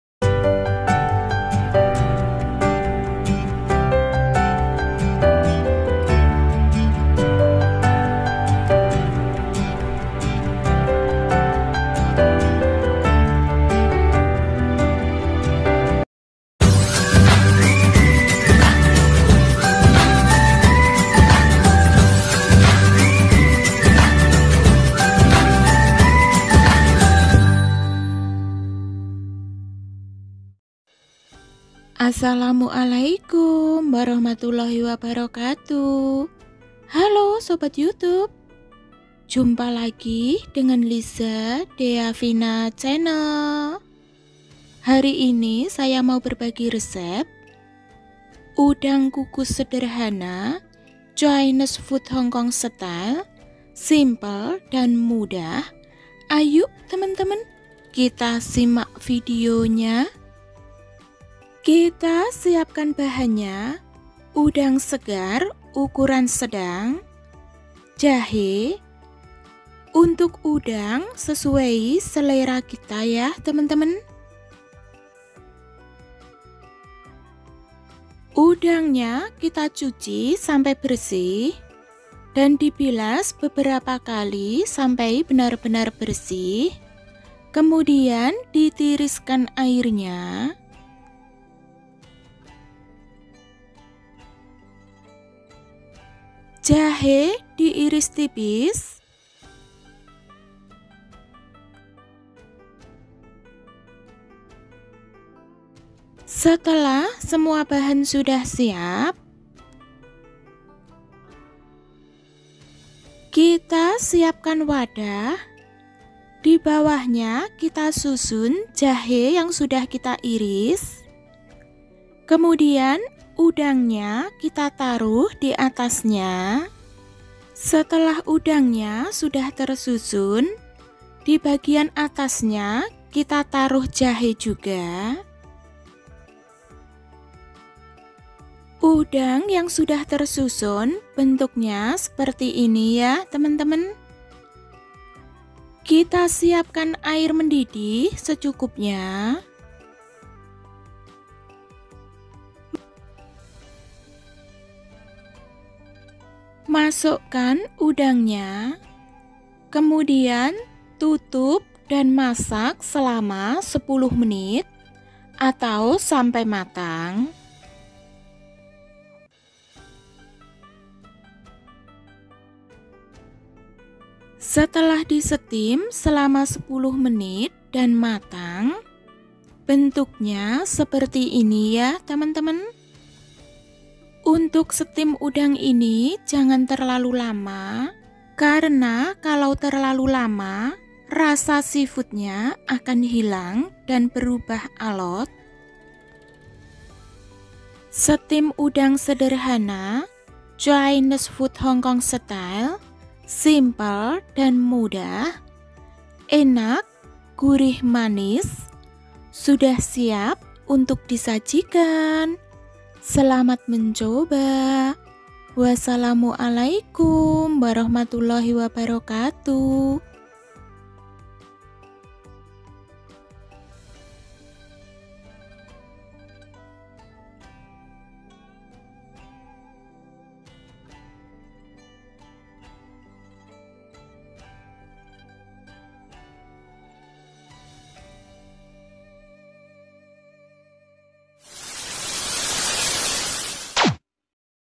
Chinese Food Recipe...!? Steamed Prawn Wit Ginger, Udang Kukus Jahe Enak dan Bergizi
Divideoku ini saya mau membagikan resep cara memasak udang kukus simple dan Istimewa manis gurih rasa seafood nya mantab chinesefood hongkong style

Bahan
Udang segar ukuran sedang
jahe

Selamat Mencoba